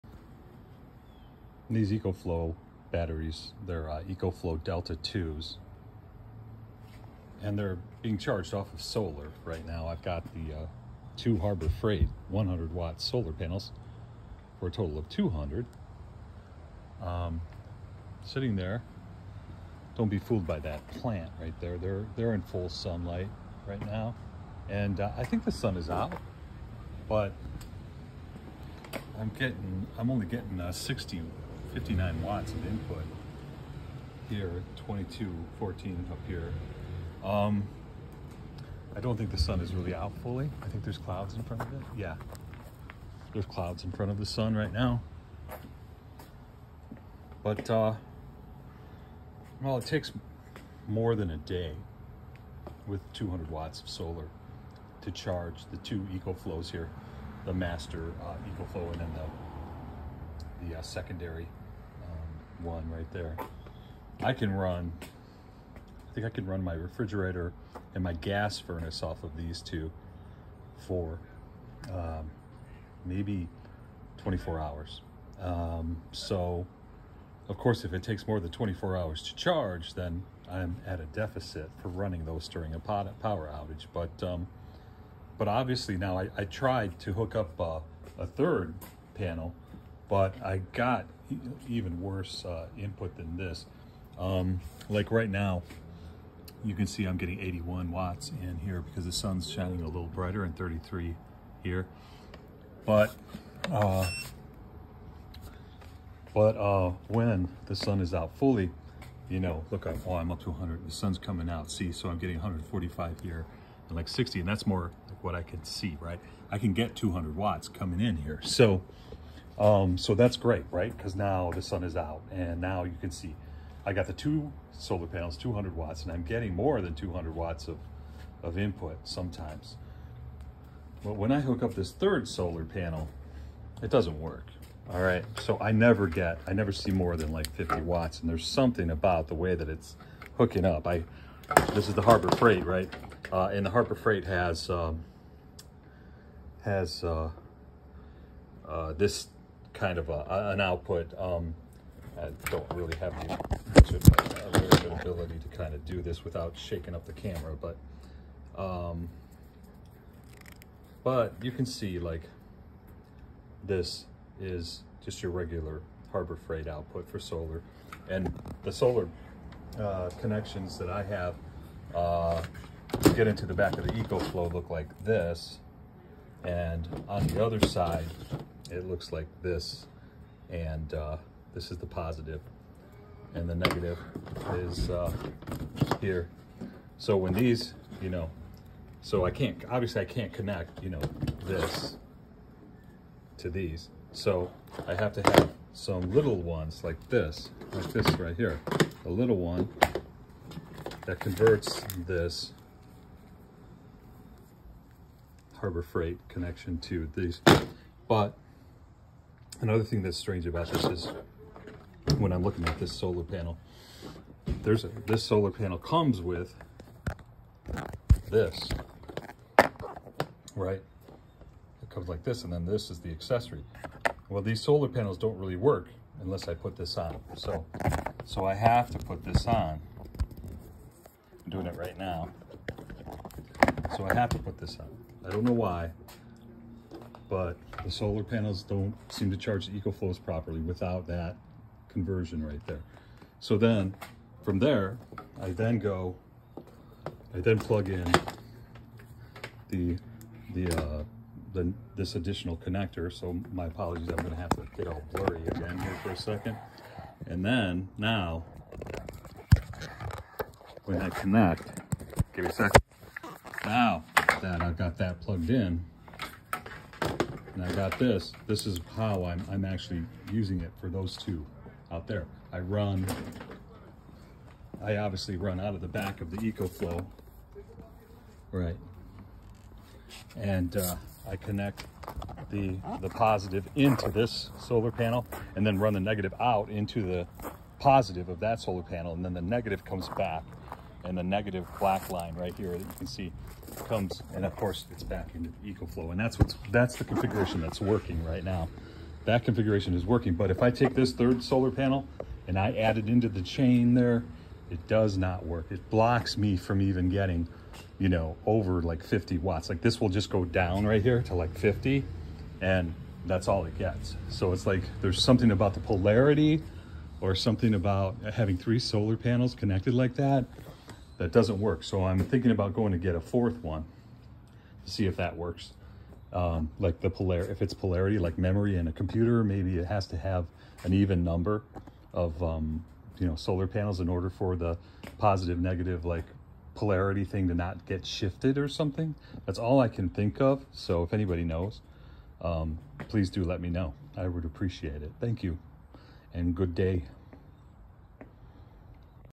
Ecoflow Delta 2 with 2x 100 W Harbor Frieght Solar Panels ?
Ecoflow Delta 2 with 2x 100 W Harbor Frieght Solar Panels.  A question about how to wire more than 2 solar panels?  Also... How to successfully wire 2 solar panels.